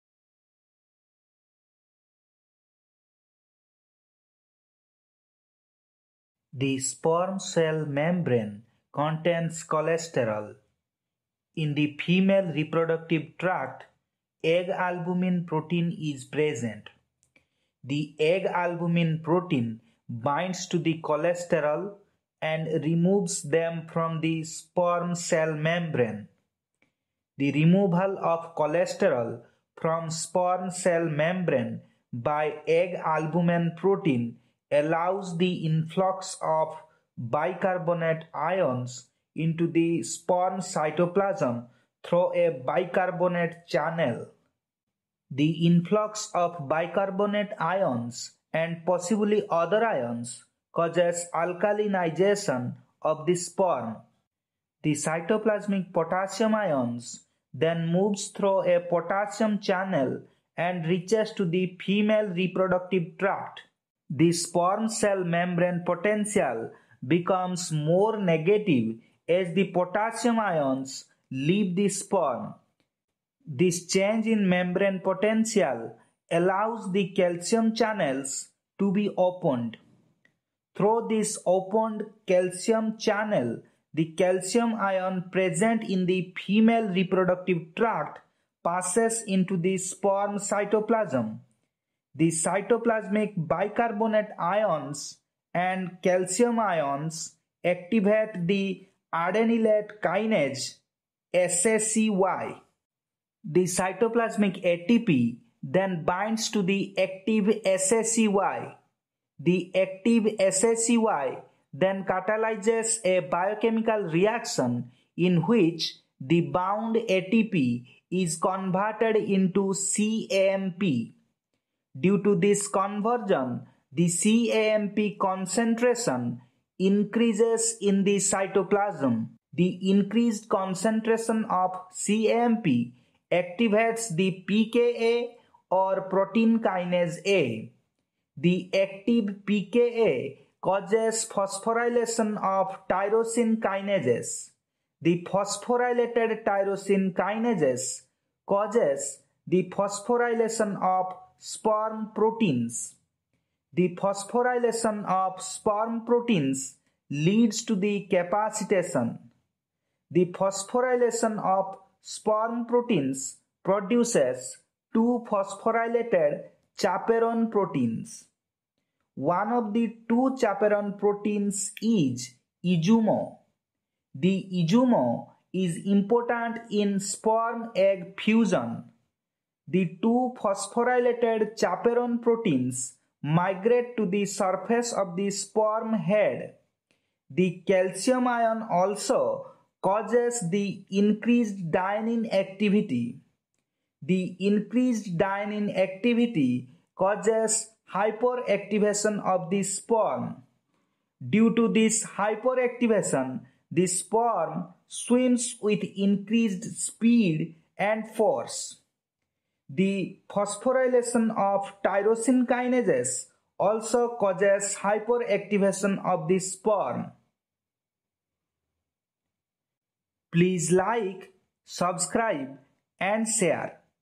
Sperm capacitation in mammal
In this video we have discussed about the hypothetical model for mammalian sperm capacitation.